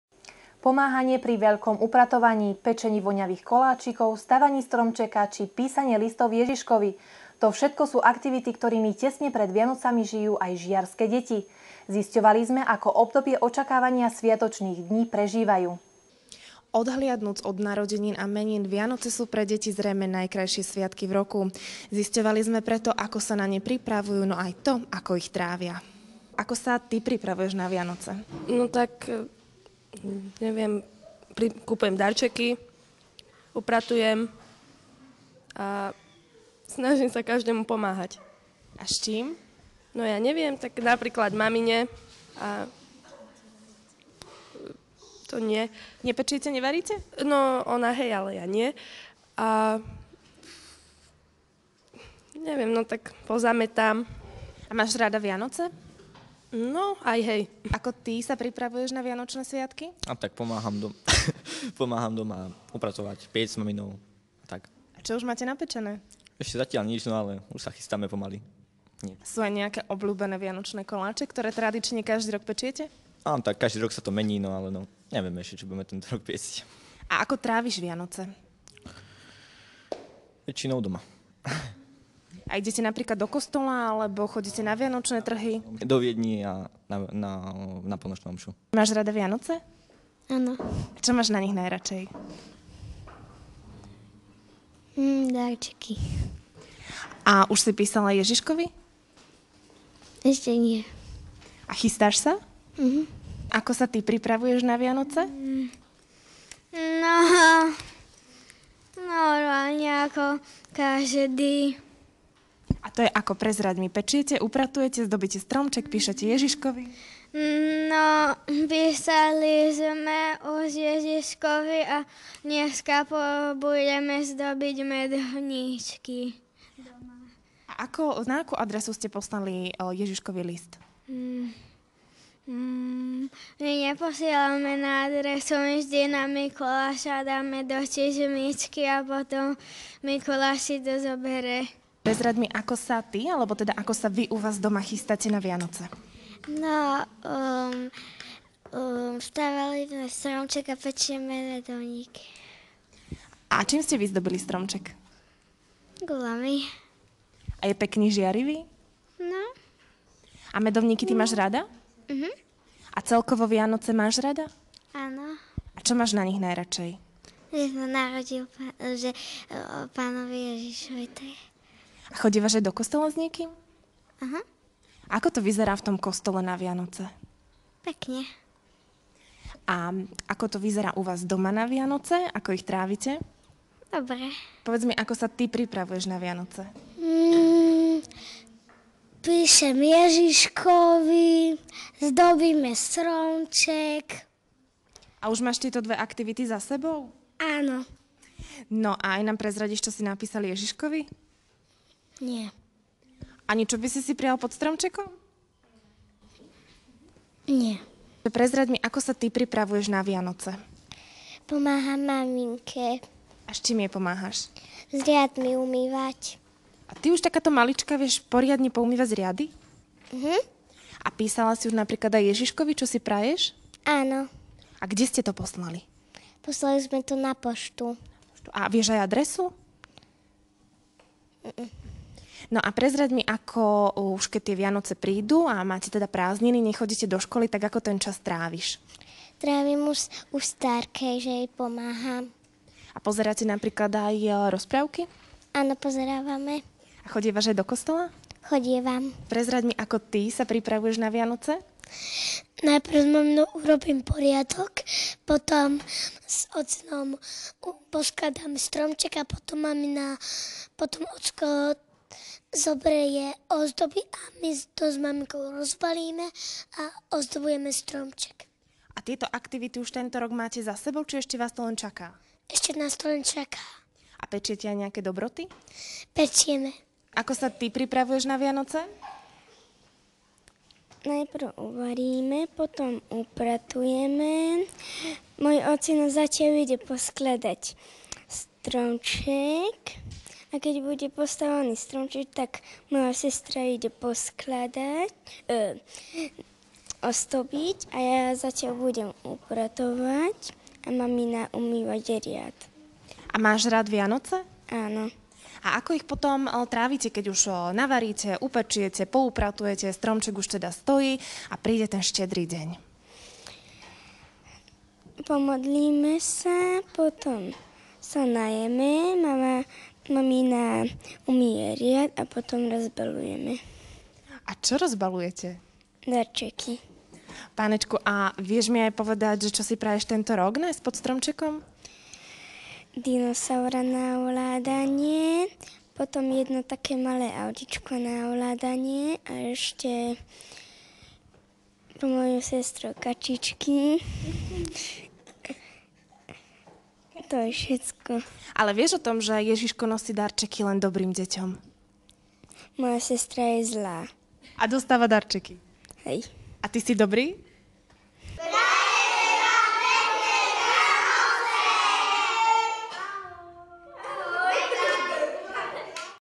Takto sa na Vianoce pripravujú deti
Pomáhanie pri veľkom upratovaní,  pečení voňavých koláčov,  stavaní stromčeka či písanie listov Ježiškovi. To všetko sú aktivity, ktorými tesne pred Vianocami žijú aj žiarske deti. Zisťovali sme, ako obdobie očakávania sviatočných dní prežívajú.